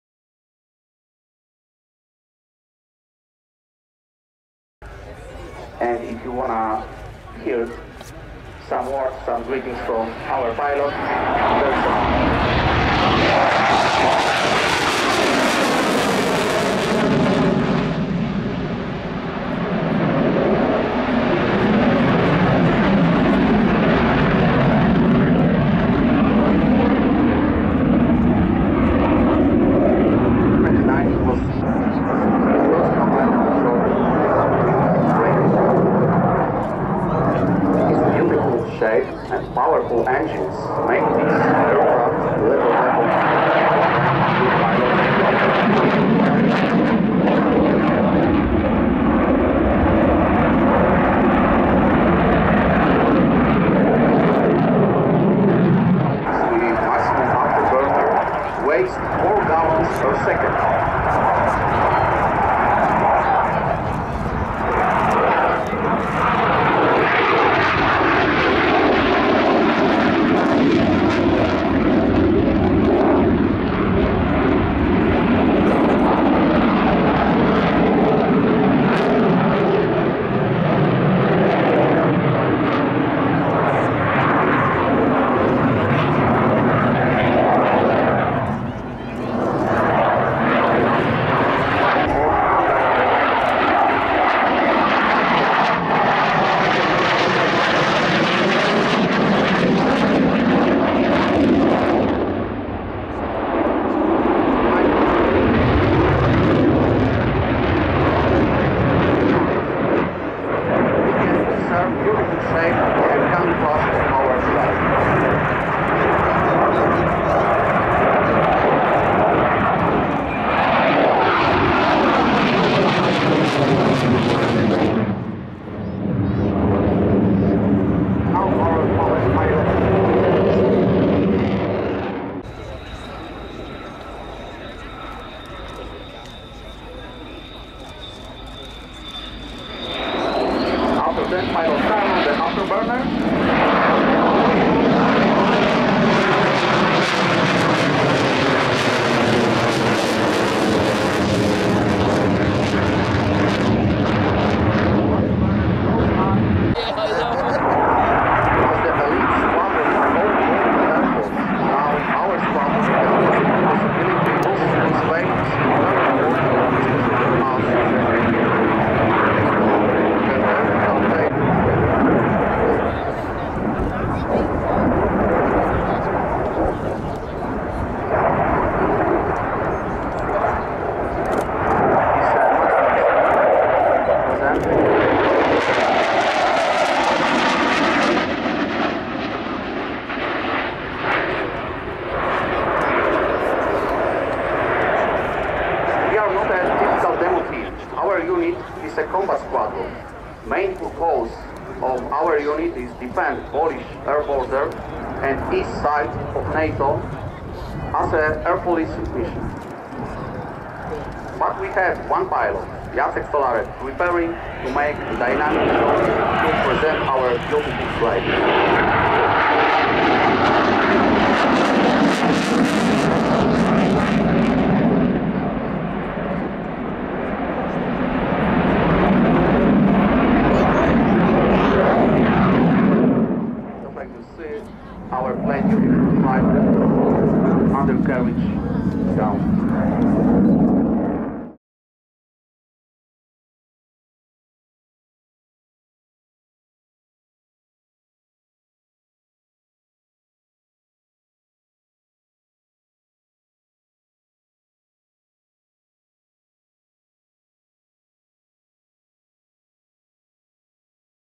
Polish Air Force MIG 29 Fulcrum at RAF Cosford Airshow 2018
MIG 29 Fulcrum display at RAF Cosford Airshow 2018